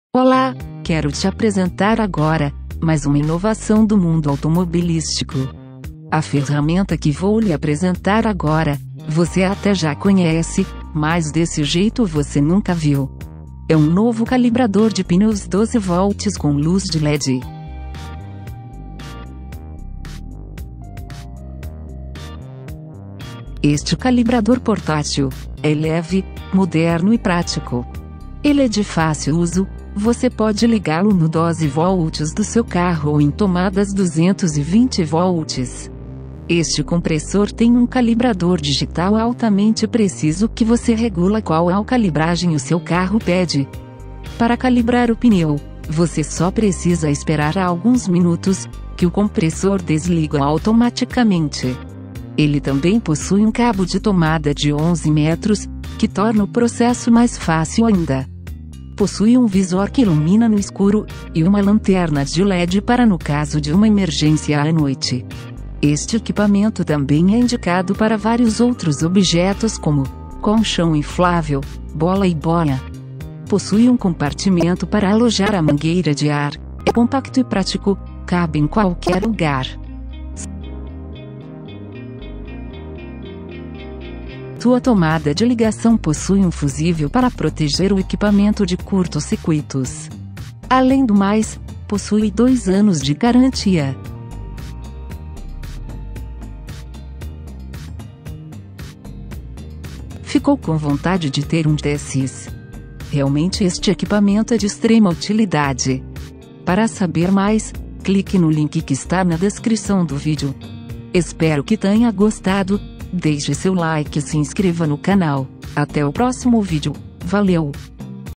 compressor portátil para calibrar pneu
calibrador portatil barato no aliexpress

Olá pessoal do canal, hoje vim trazer uma dica interessante pra vocês. Sei que todos já conhecem os compressores de ar portátil que vendem por ai, hoje estive dando uma olhada na internet e achei este compressor calibrador de pneus automático e resolvi compartilhar com vocês. É de extrema importância como todos sabem, não só encher um pneu, mas é preciso calibrá-lo corretamente, e é isso que esse equipamento faz, você não precisa ir num posto somente para calibrar um pneu, esse equipamento faz tudo pra você. Se gostarem do produto e já quiserem adquirir o link está na descrição do vídeo.

compressor portátil
compressor portátil 12v
calibrar pneu
compressor
 compressor de ar portátil com calibrador